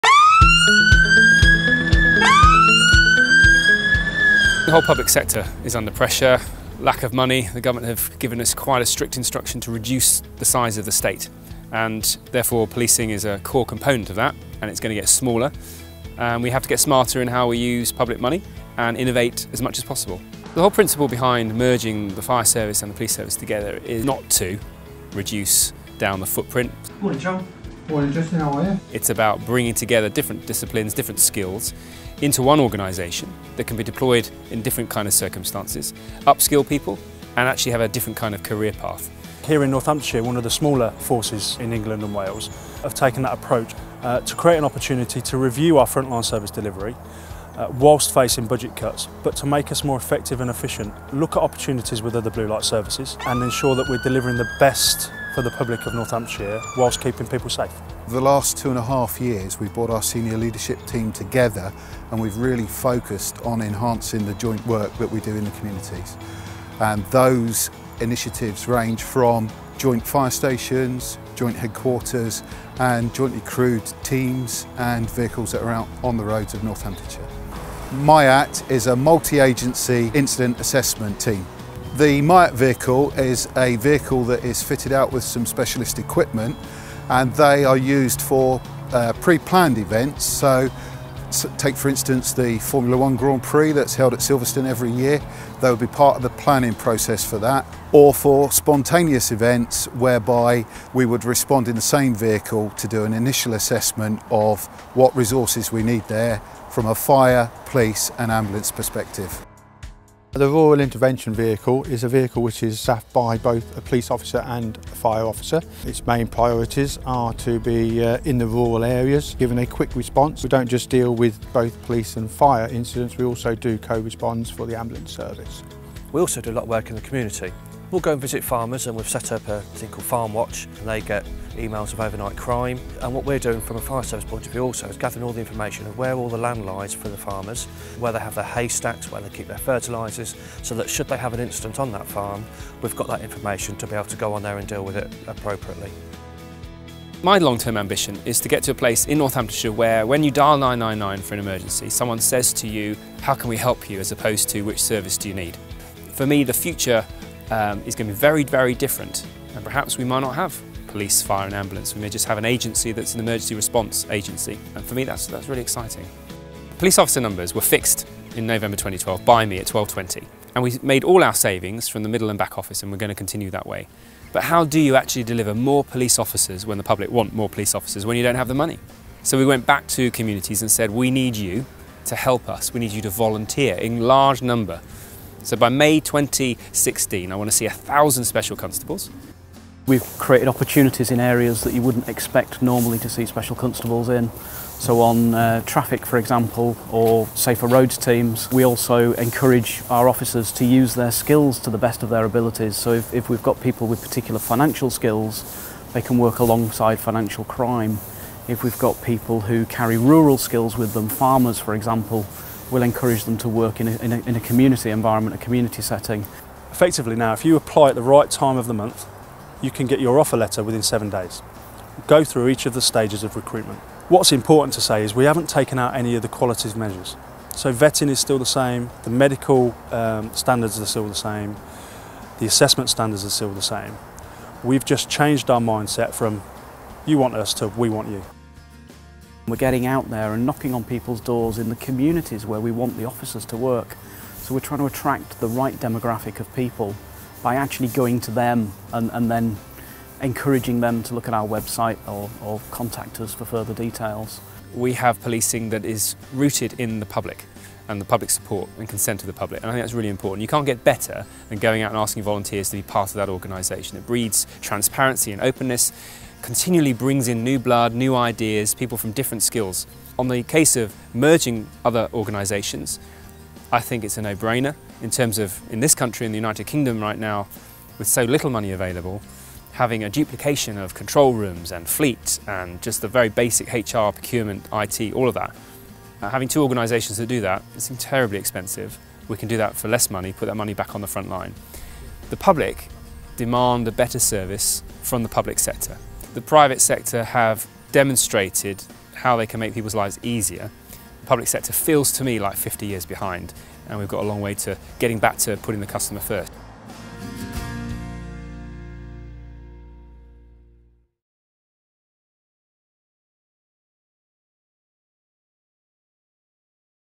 Northamptonshire Police Force
Northamptonshire Police is a forward looking force which is seeking to utilise its resources in the most innovative ways possible at a time when budgets are seriously under pressure.
The force is increasing role of volunteers and Special Constables within Northamptonshire Police. At the same time it’s moving towards joint working with Northamptonshire Police and the Northamptonshire Fire and Rescue Service.